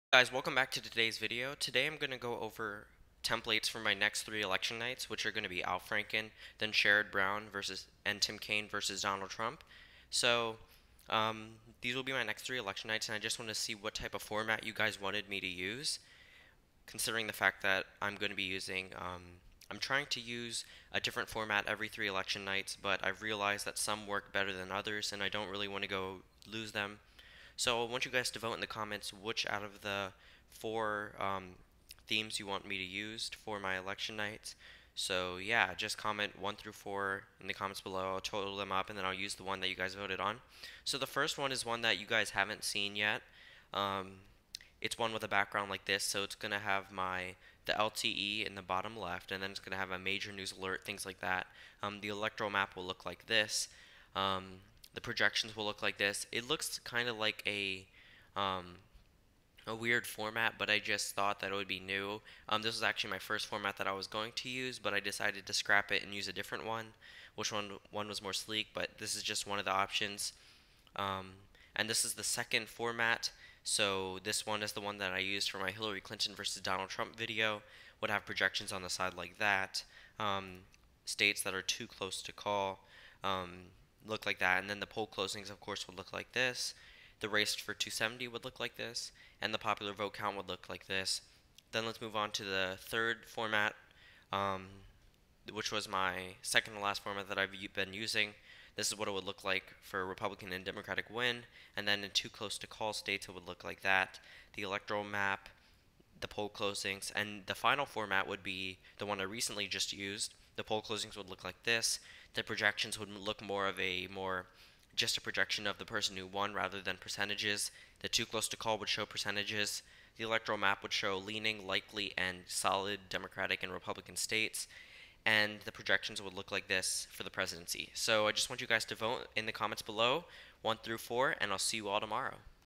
VOTE ON THE THEME FOR NEXT 3 ELECTION NIGHTS
Vote in the description what format you'd like to see for my next 3 election night videos (1-4)!